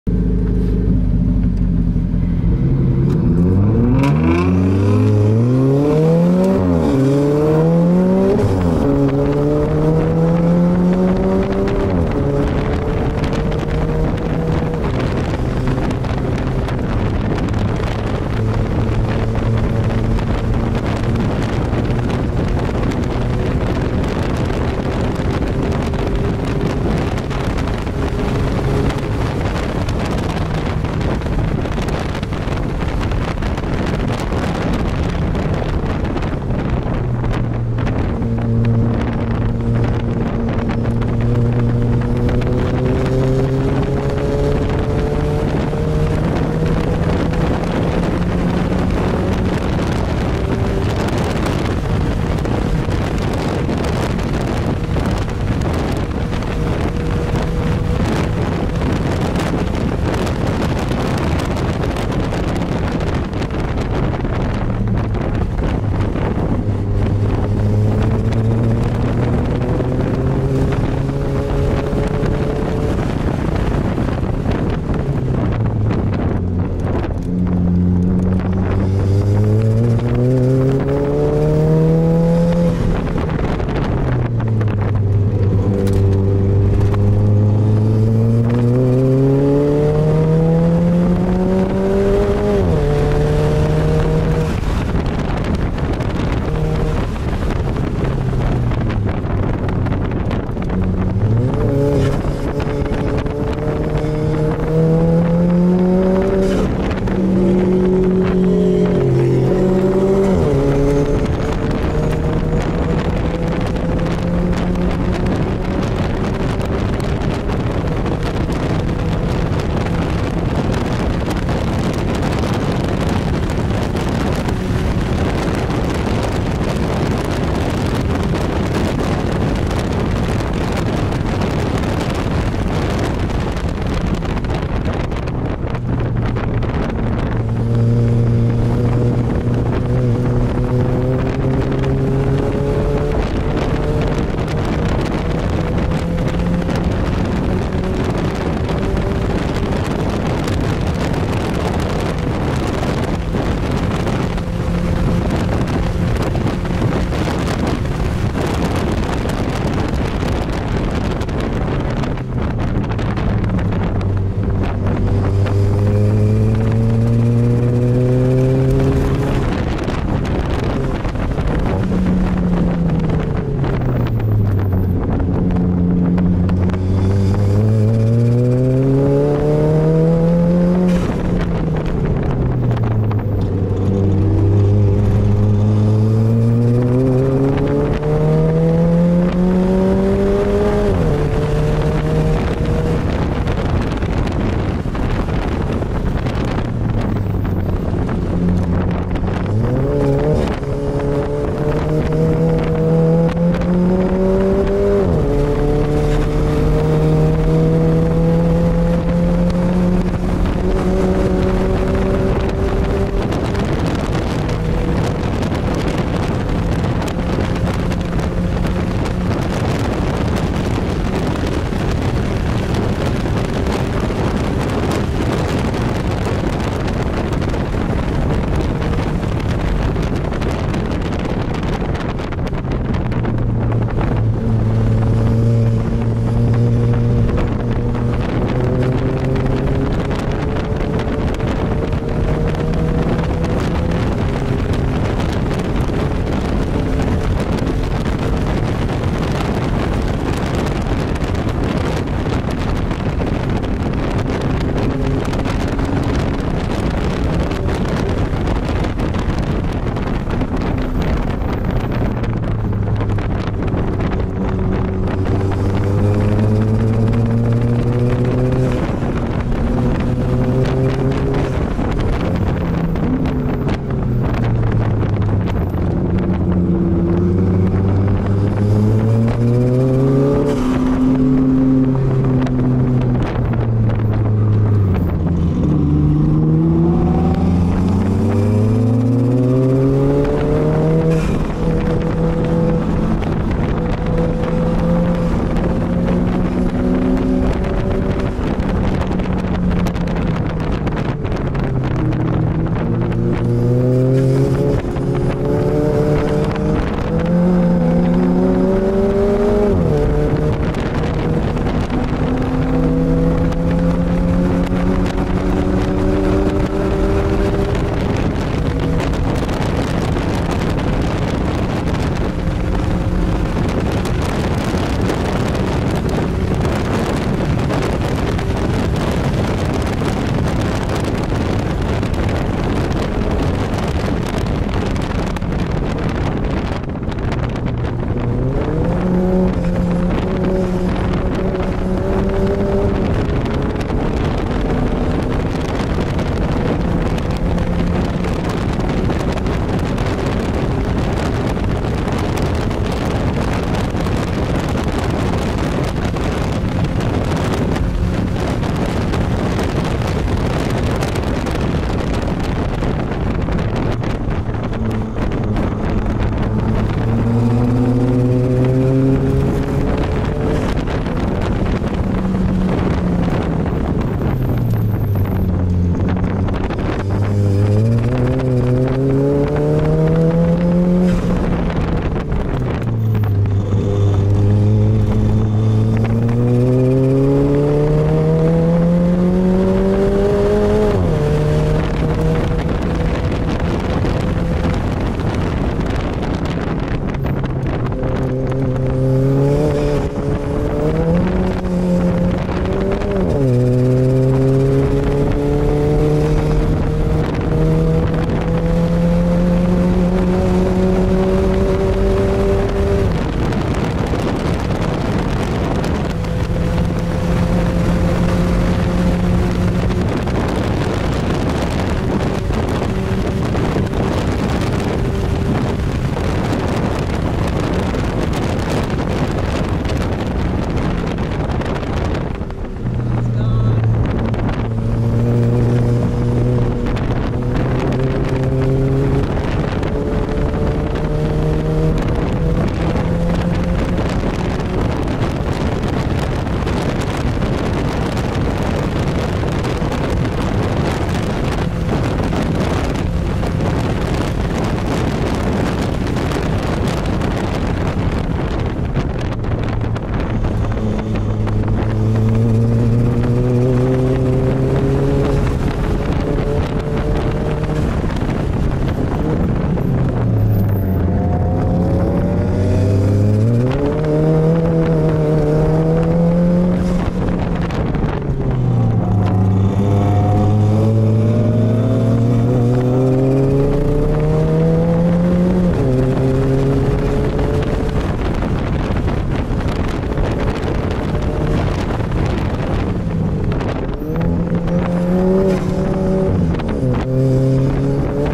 Alfa Romeo 4C in Race mode at SCR with GTR inside car ActionCam video.
Inside car Action Cam video. He is good driver and driving his nice car very good, its getting harder!